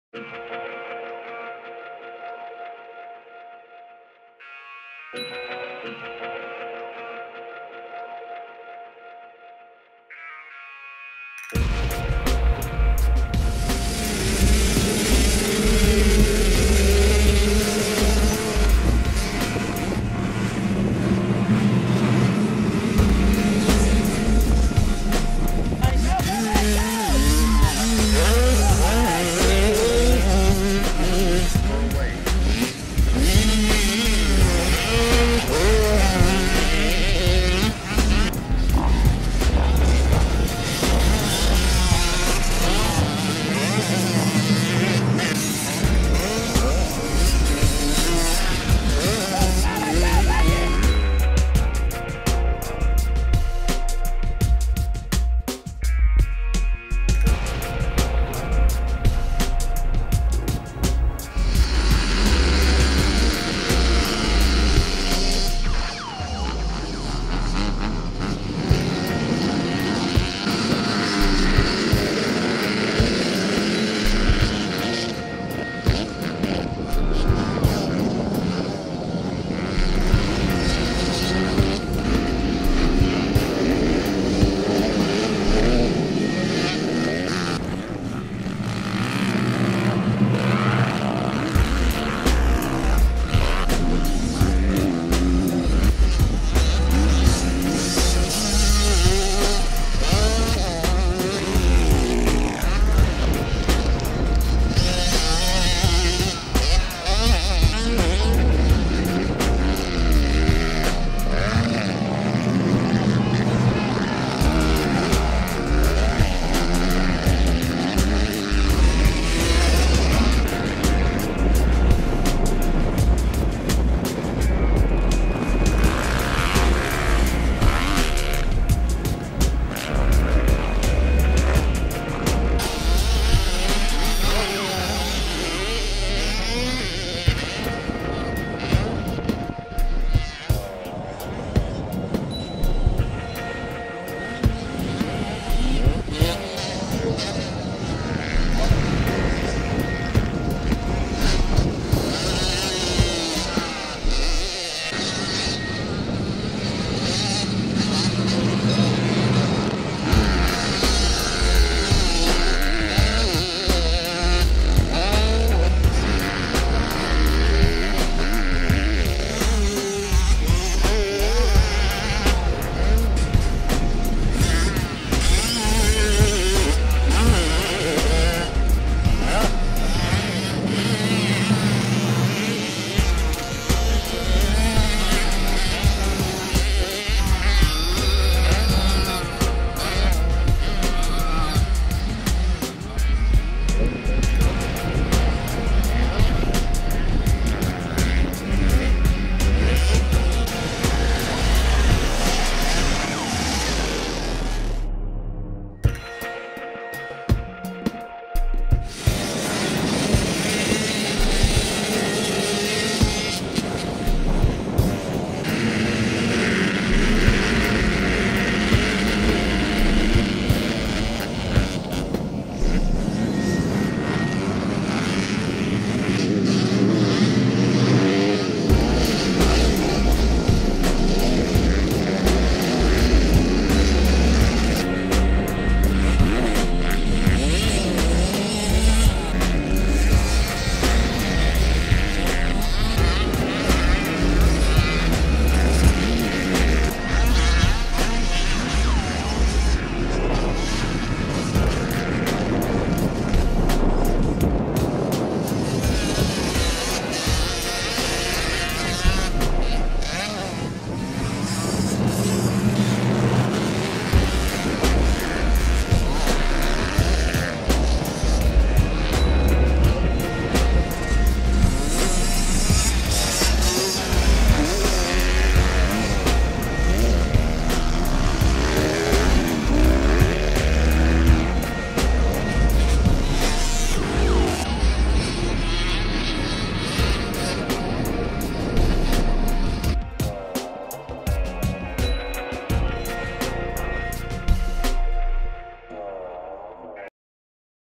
ADVMX Race Highlights 85/150/Schoolboy 11/13/11
Youth Race Highlights from Austin Del Valle Motocross Park Race 85 open, 150 Open, 150 Beginner, and Schoolboy.  Evolution Series November 13, 2011.  These kids are on 85 cc 2 stroke, 150 cc four stroke, super mini 2 stroke, and 125 2 stroke and 250 four stroke motocross race motorcycles.  Some of the riders here include:  48 Lane, 182 Hudson, 86 Brandon W, 51 Brandon M, 818 Tanner, 75 Hunter, 876 Oliver B, 222 Dylan W, 78 Gabe, 186 Devlin W, 142 Bailey P, 28 Dalton, 272 Jacob H, 26 Mason C, 242 Colton H, 7 Cale G, 28 Bailey R  Please message us if you know more.